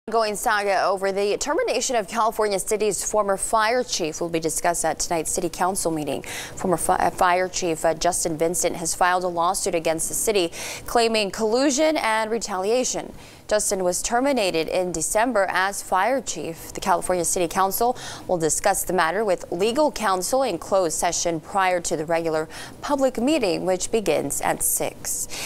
Ousted fire chief to be discussed during Cal City city council meeting
Former Chief Justin Vincent has filed a lawsuit against the city, claiming collusion and retaliation.